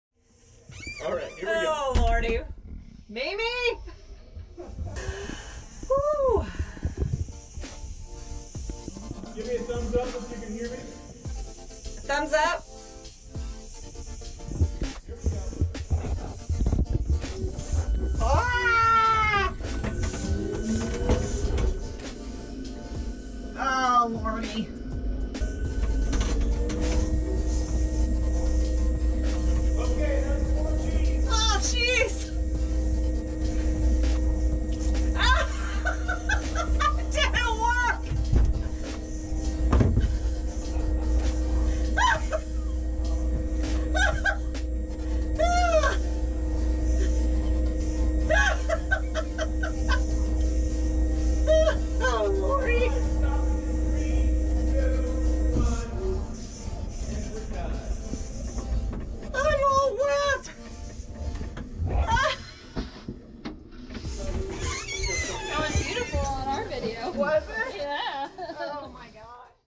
G Forces
What happens if you try to drink in a 4G centrifuge?